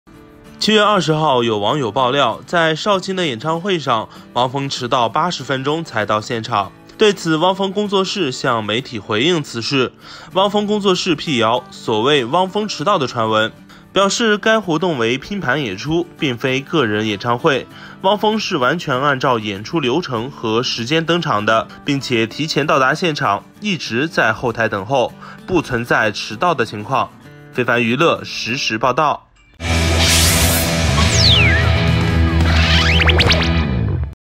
【當月】汪峰演唱會遲到引熱議 工作室辟謠：完全按照時間登臺！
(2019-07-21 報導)

7月20日，有網友曝料在紹興的演唱會上，汪峰遲到80分鐘才到現場。對此，汪峰工作室向新浪娛樂獨家回應此事。汪峰工作室辟謠所謂“汪峰遲到”的傳聞，表示該活動為拼盤演出，并非個人演唱會，汪峰是完全按照演出流程和時間登臺的，并且提前到達現場，一直在后臺等候，不存在遲到情況。